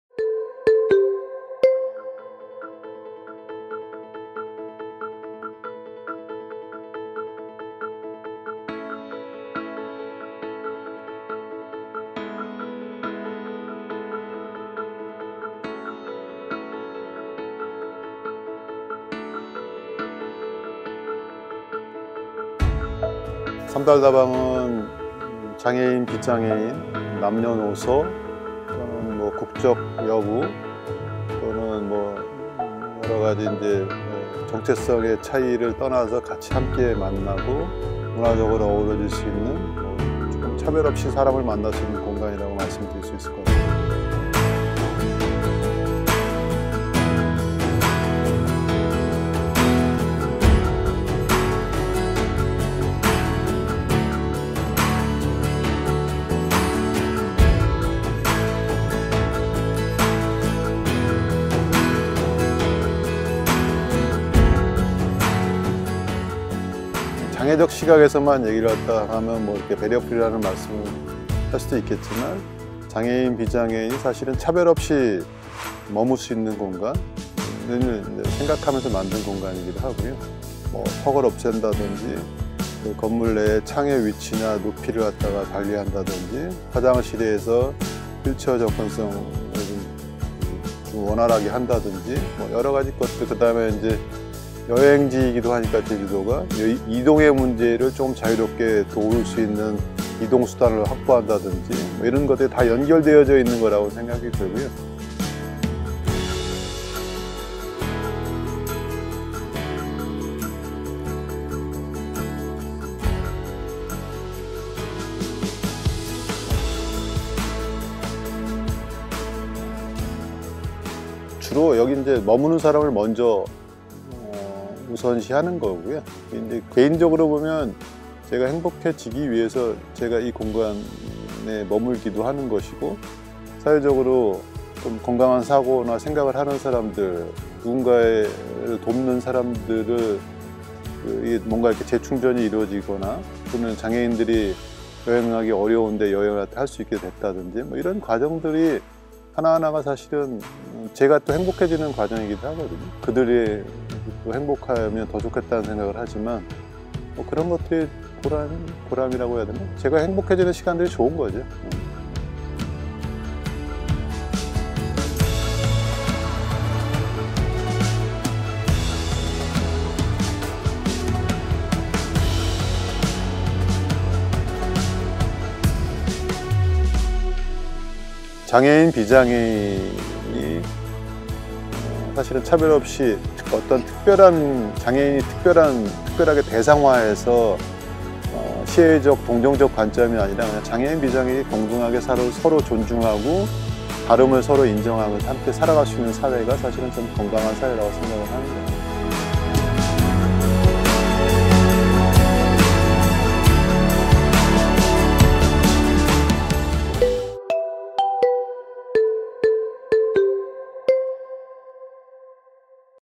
[BOKA NEWS_53] 제주 배리어프리 숙소 삼달다방
모두가 여행하고 싶은 제주도.
하지만 제주여행이 여의치 않았던 사람들도 있습니다.
그들을 위해 성산읍 삼달리에는
제주를 즐기며 머물 수 있는 특별한 공간이 있는데요.

바로 '삼달다방'입니다.
이 곳은 배리어프리 숙소로
장애의 여부와 상관없이 누구에게나 문이 활짝 열려있습니다.

이곳에서 어떤 배리어프리 요소들이 있는지
보카가 '삼달다방' 이상엽 대표를 통해
자세한 이야기를 만나봤습니다.

'보카가 전해드릴게요. 따뜻한 제주 이야기.'

editor. 현규리
producer. 김효성, 이준석
computer graphics. 최수빈
👉 제주보카 SNS바로가기